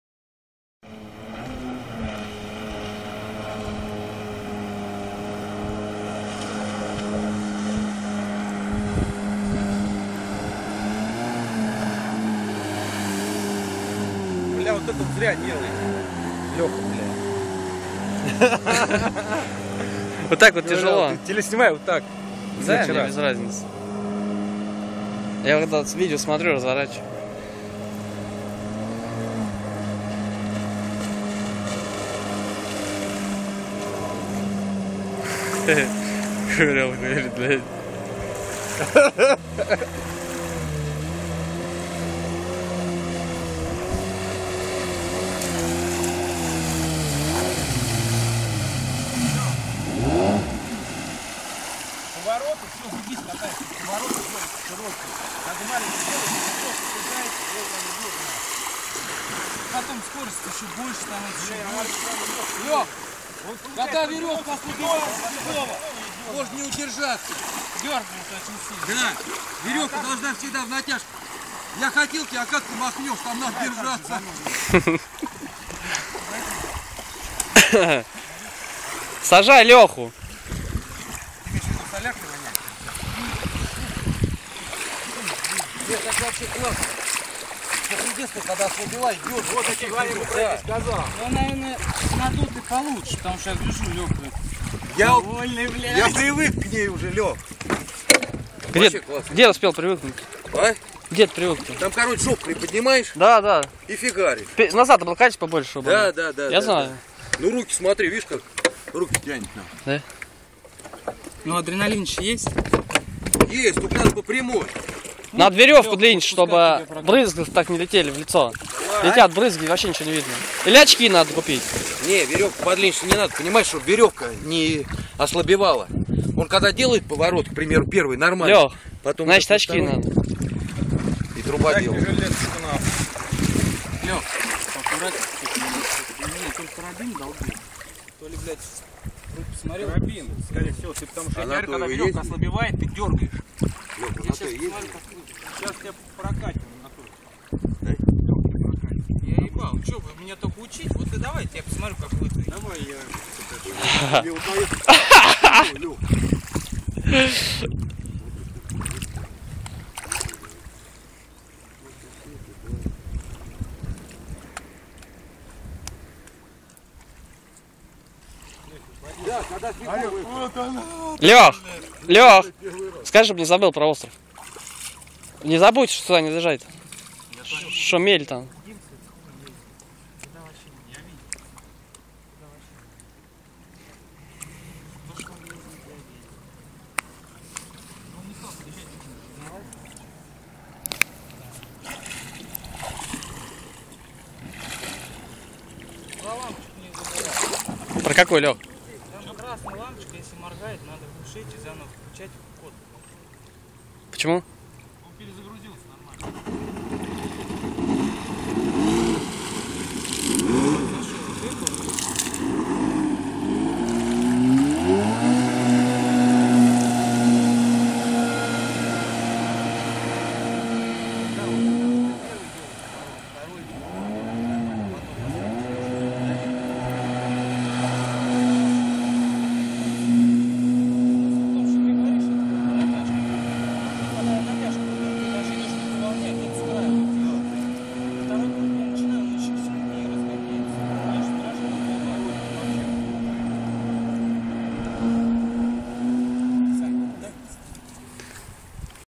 гидроцикл ямаха
Река ока, отдых на
Гидроцикл  Yamaha 800 с друзьями- хорошо провели время использовали для катания ватрушку китайского производства, на которой кататься было комфортно с весом тела до 85 килограмм, так что приобритайте ватрушки более дорогие и не портите себе удовольствие от катания река ока гидроцикл прокатгидроциклов ватрушка Гидроцикл  Yamaha 800